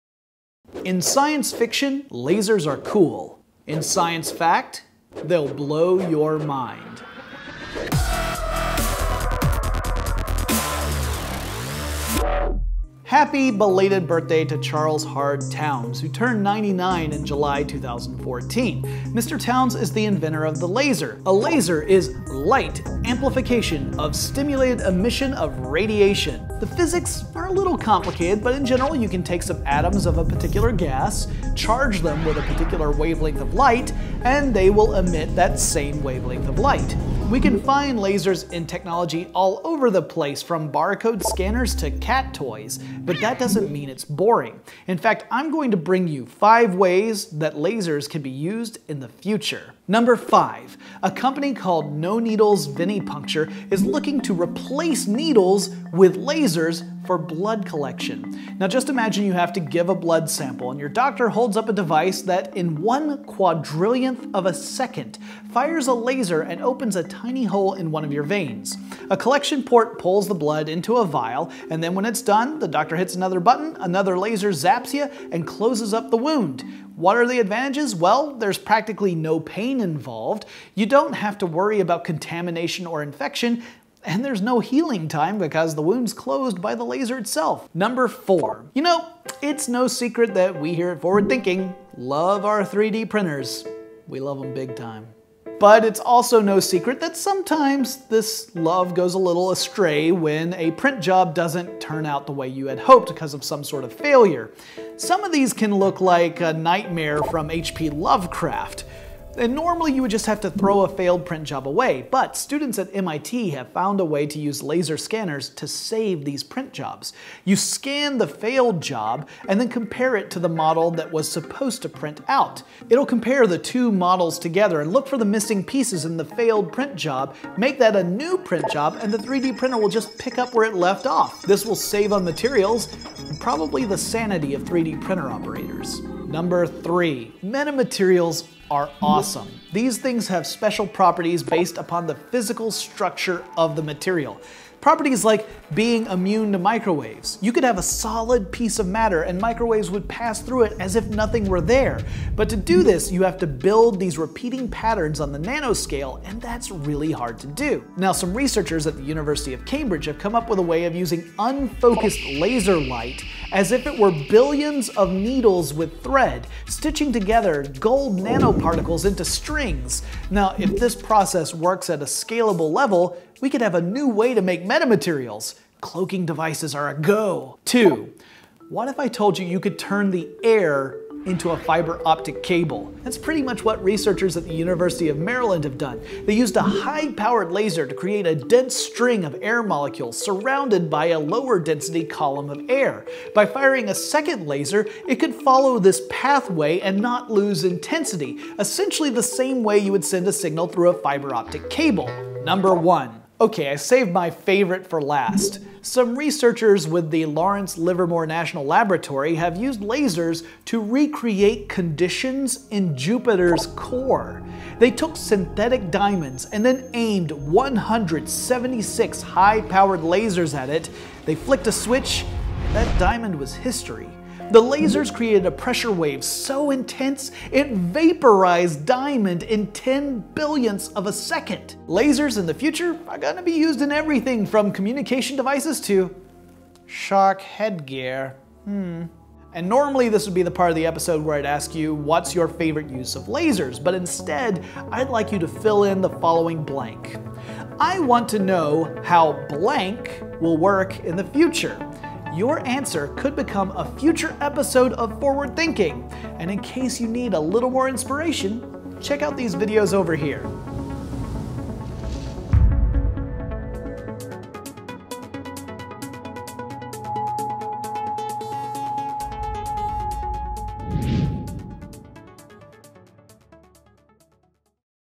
5 Ways Lasers Will Be Used in the Future
Happy belated birthday to Charles Hard Townes - Nobel Prize-winning physicist and inventor of the laser. In honor of Mr. Townes, we’re talking about five mind-blowing ways laser technology will be used in the future!

Have a topic you’d like us to cover? Give us your ideas in the comments below! Your response could become a future episode of Fw:Thinking!